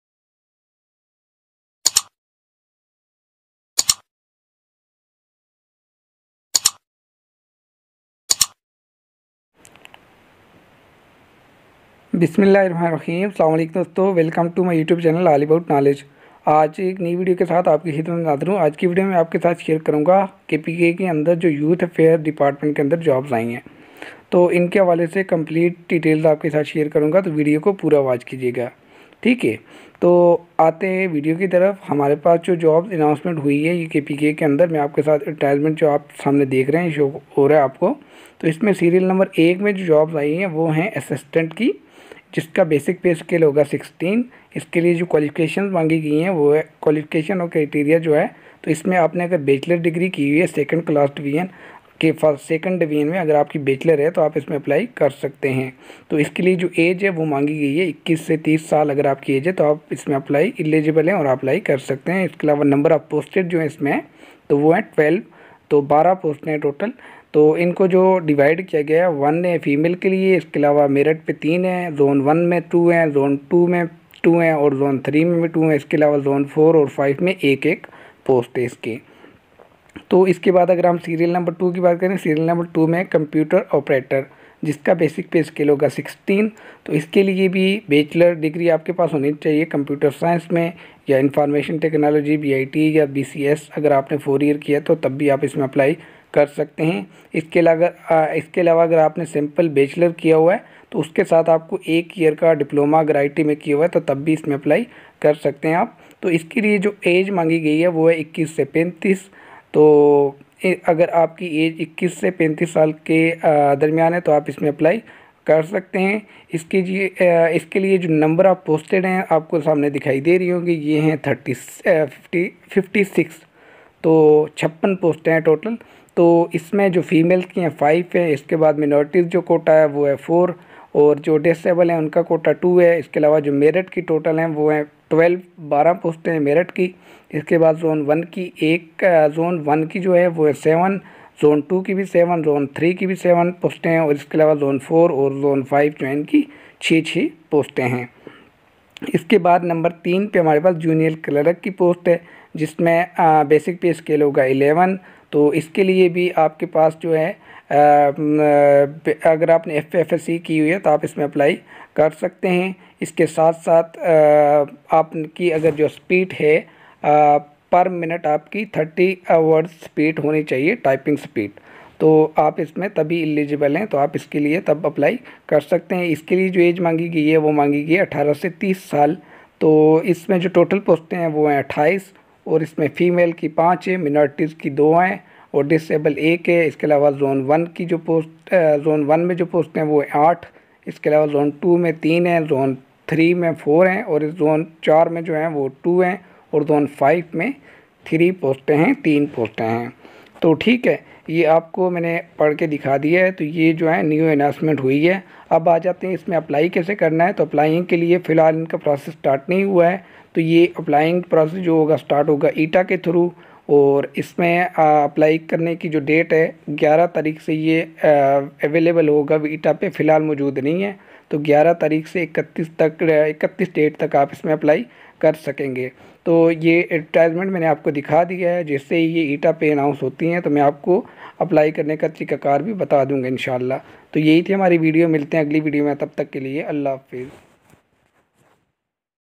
Youth Affairs department!jobs 2022 jobs in kpk.
Government of Khyber pakhtoon khawa
New govt jobs in directorate of youth Affairs department Khyber pakhtoon khawa.

Online  process will be start from 11 of January to 31 January through Etea official.